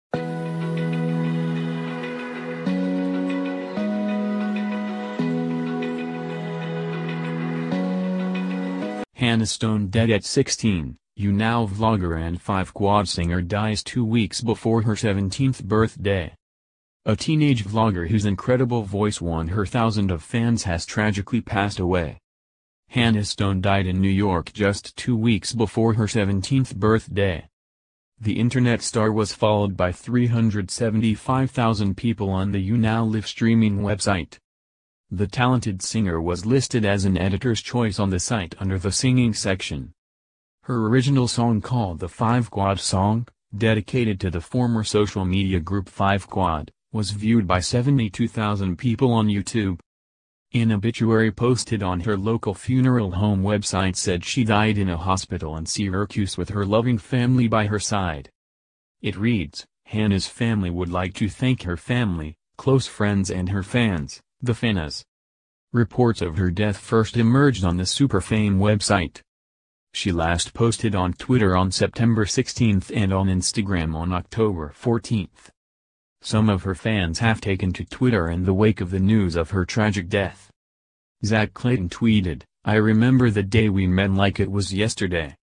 Hannah Stone dead at 16 – YouNow vlogger and 5quad singer dies two weeks before her 17th
Hannah Stone dead at 16 – YouNow vlogger and 5quad singer dies two weeks before her 17th birthday.
A TEENAGE vlogger whose incredible voice won her thousand of fans has tragically passed away

Hannah Stone died in New York just two weeks before her 17th birthday.

The internet star was followed by 375,000 people on the YouNow live streaming website.